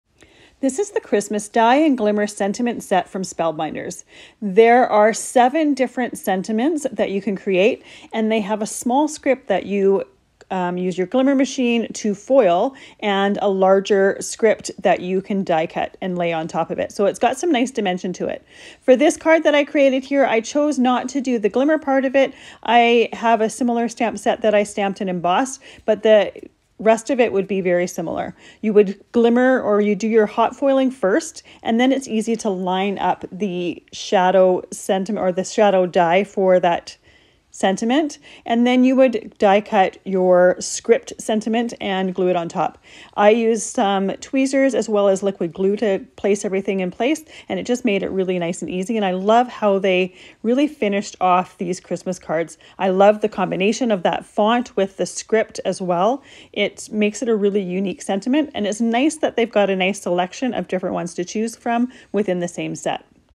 Spellbinders Christmas Die & Glimmer Sentiments Review
I really love how the Spellbinders Christmas Die & Glimmer Sentiments combine both die-cut and glimmer fonts in this set.